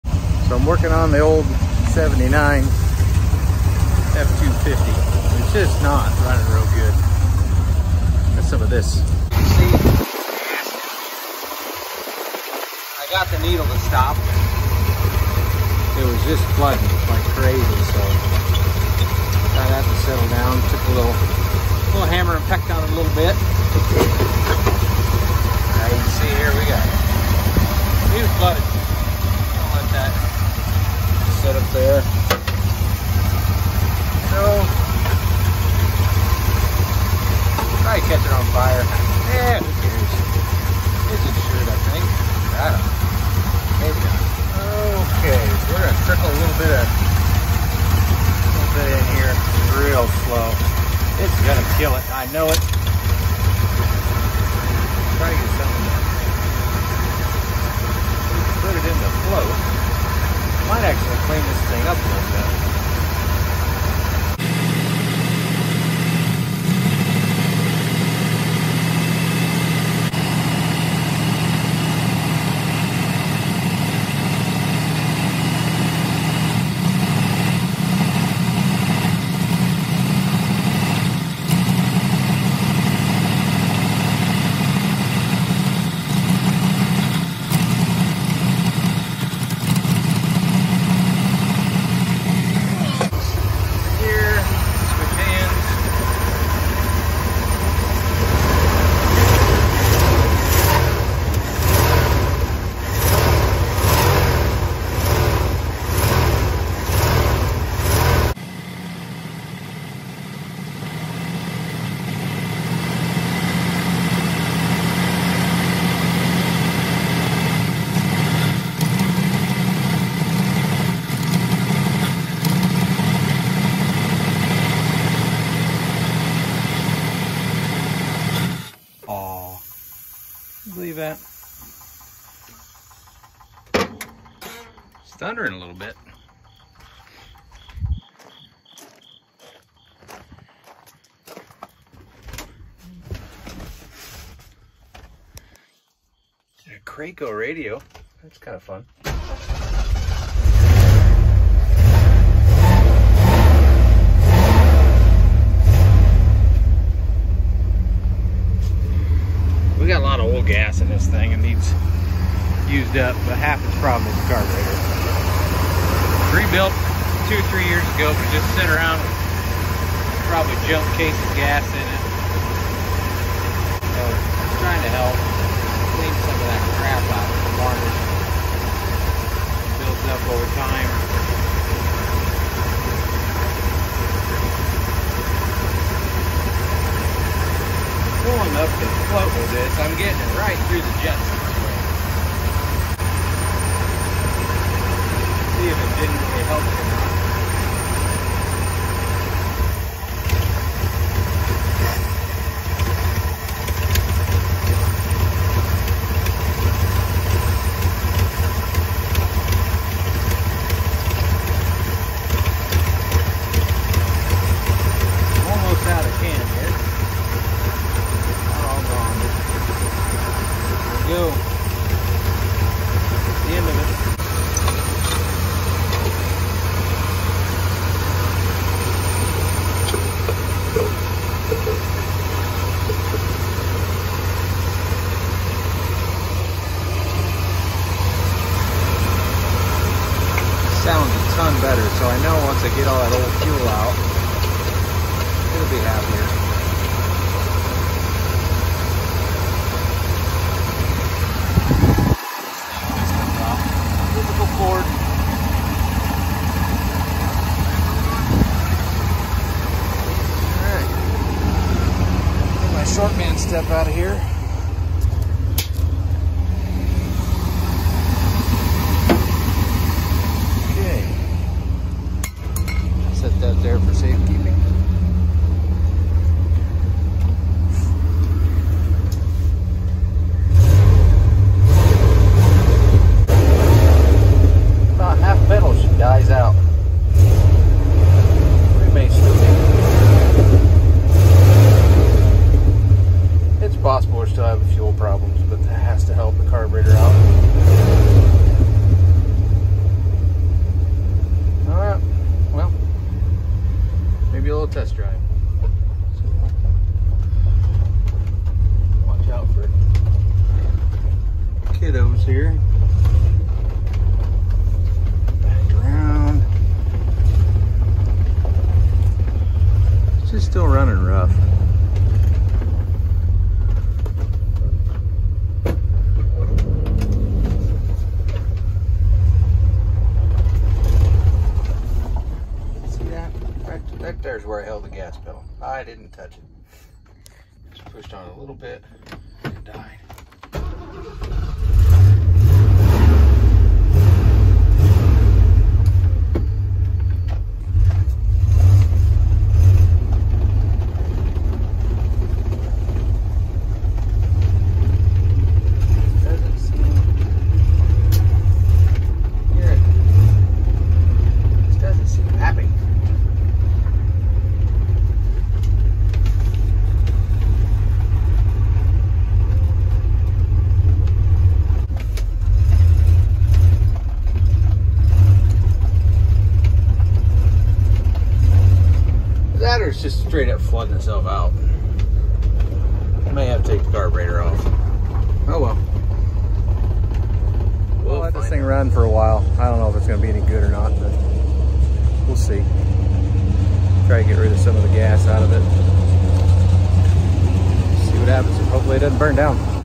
October 7, 2021
F250 gets a tune up from a can of Berrymans